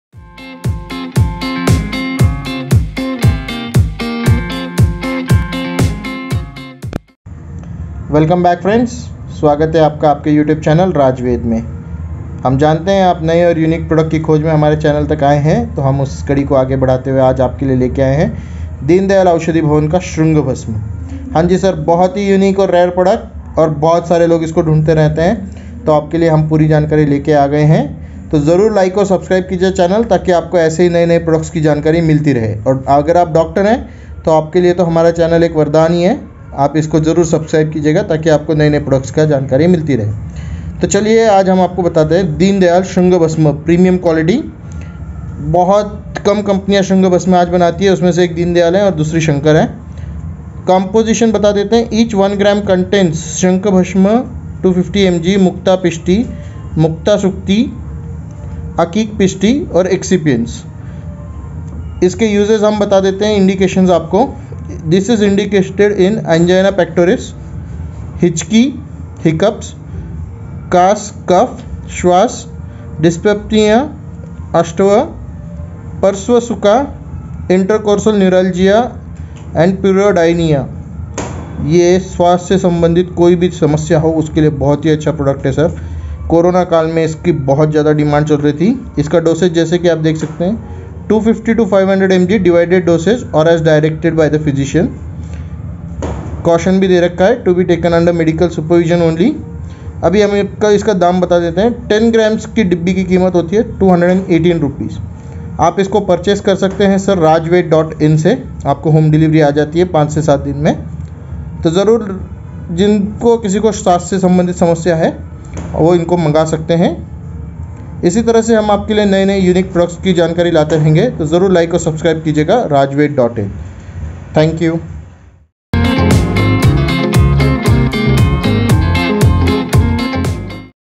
Ayurvedic medicine of heartburn | shring bhasma | dindayal | indication | uses | dosage | benefit
DESCRIPTION :
XXX
INDICATION :
useful in
angina pectoris
hichaki, kasa
cough, svasa
urastoya, kasa
XXX
COMPOSITIONS :
each 1gm contains :
shankha bhasma
mukta pishti
mukta shukti
akik pisti
XXX
DOSAGE :
250 to 500mg divided dosage
or as directed by
the physician
XXX
OTHER BASIC INFORMATION :
SHRINGA BHASMA (PREMIUM)
DINDAYAL AYURVED BHAVAN
PACK SIZE : 10GM
PACK PRICE : 216.00rs
EXPIRY :  3 years from manufacuring date